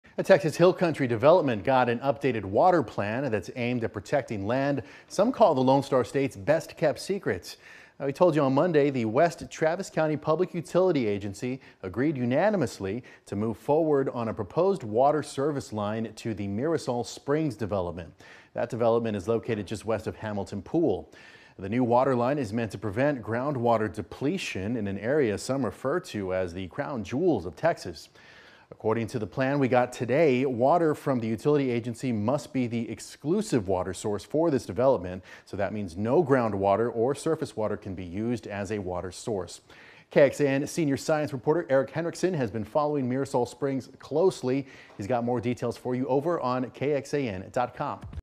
Updated water plan for Hill Country development meant to protect Texas’ ‘crown jewels’
After several months, the West Travis County Public Utility Agency (WTCPUA) released the updated water plan for a Texas Hill Country development on Wednesday morning.